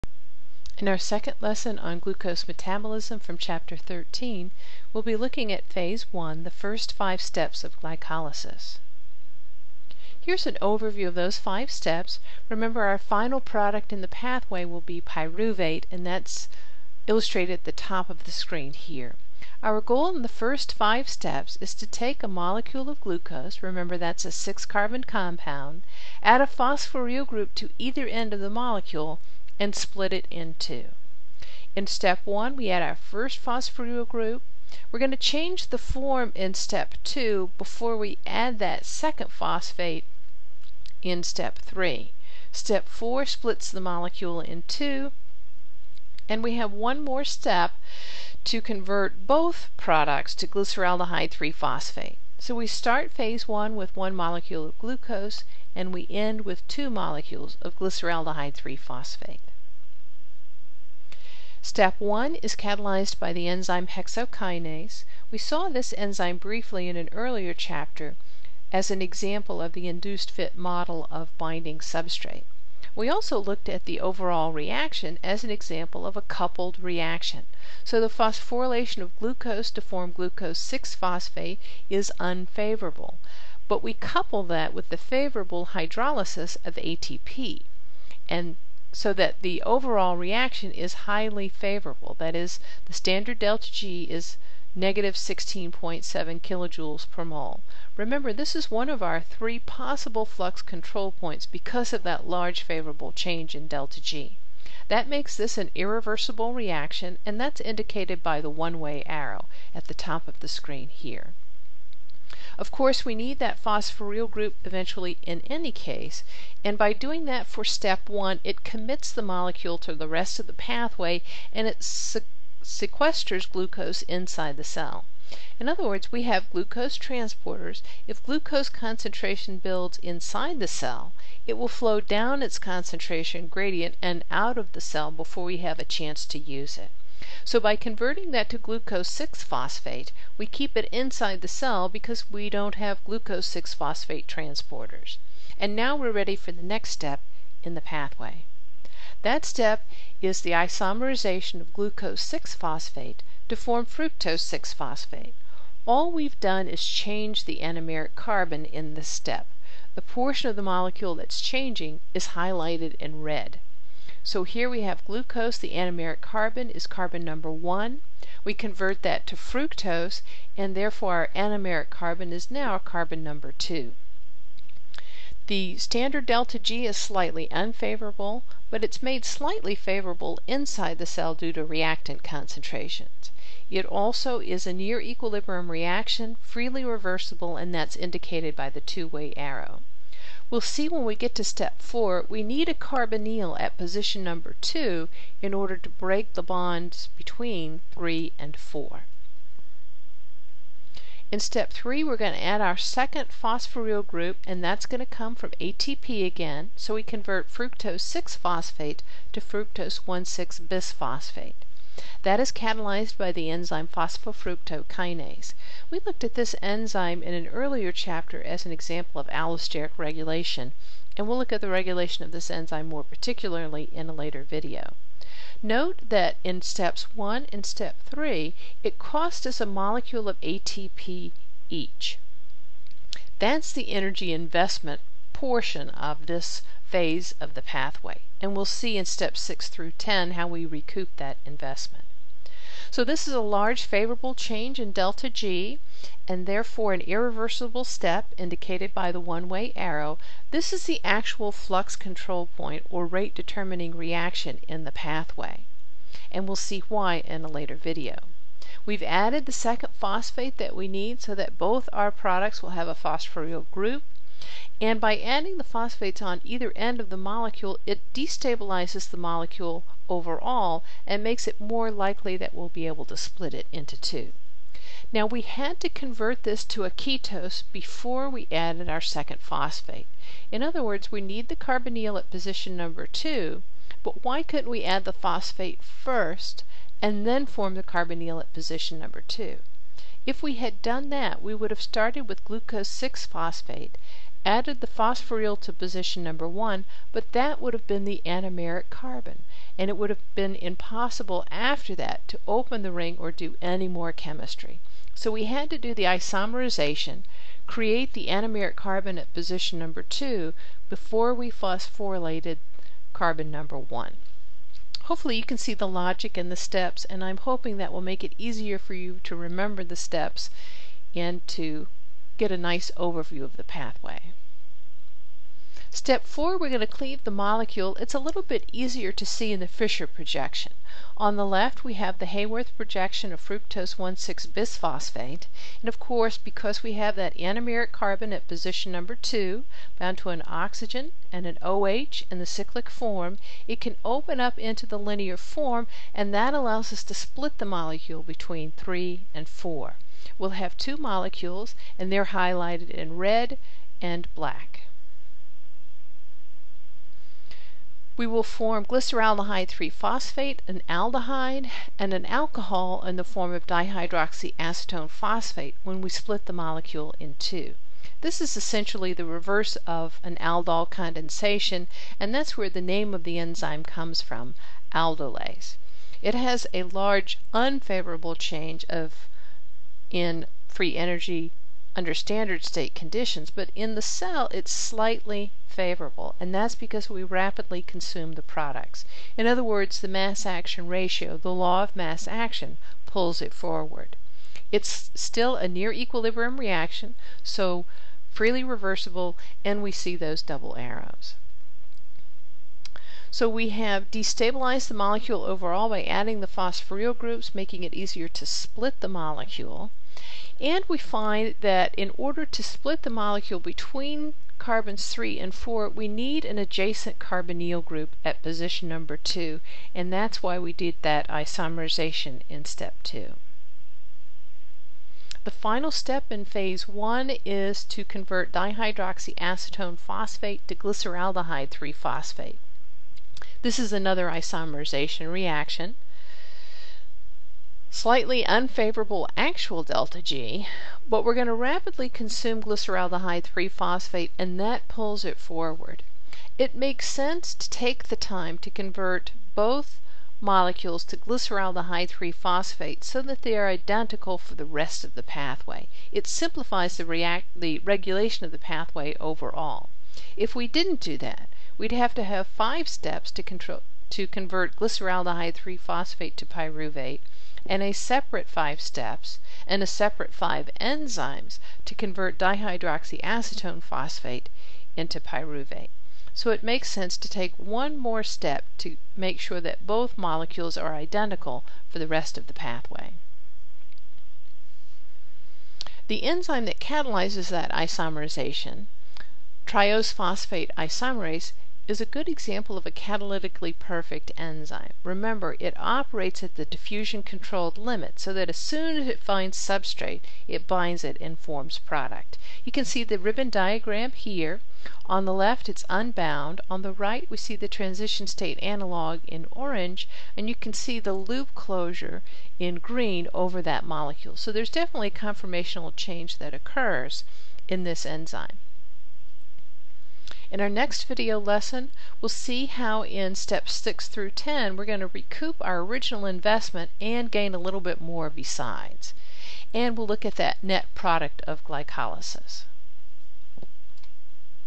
092-Phase 1 of Glycolysis
Detailed review of steps of energy investment phase of glycolysis